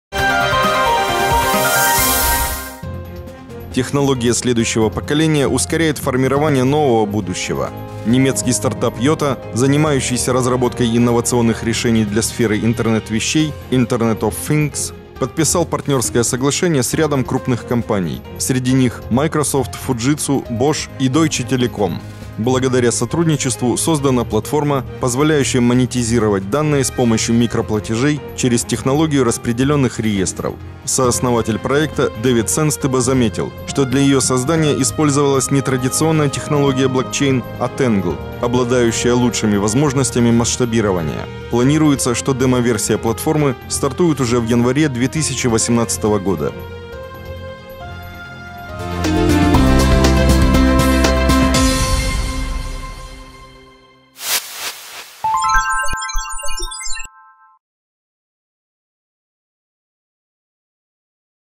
Создана инновационная платформа для Интернет вещей
Технология следующего поколения ускоряет формирование нового будущего. Немецкий стартап IOTA, занимающийся разработкой инновационных решений для сферы интернет вещей (IoT - Internet of Things), подписал партнерское соглашение с рядом крупных компаний, среди них: Microsoft, Fujitsu, Bosch и Deutsche Telekom. Благодаря сотрудничеству создана платформа, позволяющая монетизировать данные с помощью микроплатежей через технологию распределенных реестров. Сооснователь проекта, Дэвид Сенстебо заметил, что для ее создания использовалась не традиционная технология блокчейн, а Тэнгл (Tangle), обладающая лучшими возможностями масштабирования. Планируется, что демоверсия платформы стартует уже в январе 2018 года.